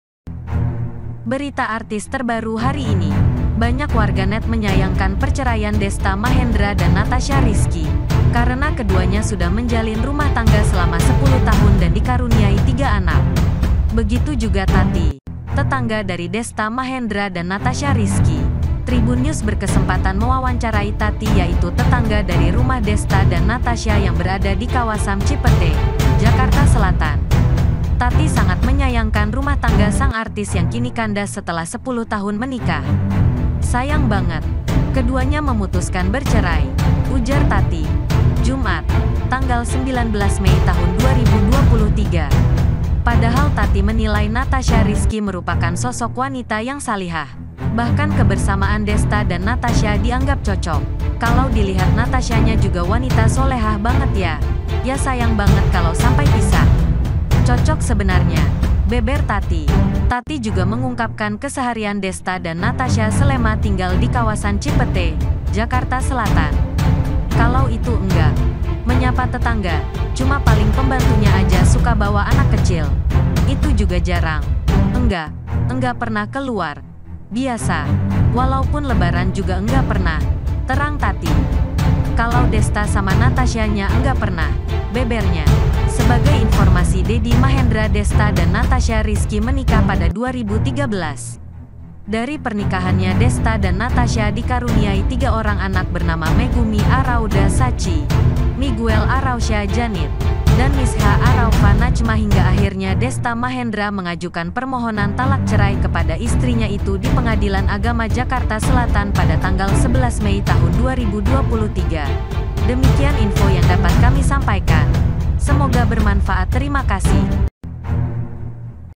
TETANGGA DESTA MENYAYANGKAN PERCERAIAN - berita selebriti terbaru hari ini
yuk ikuti terus berita selebriti terbaru hari ini
agar tidak ketinggalan berita menarik lainnya.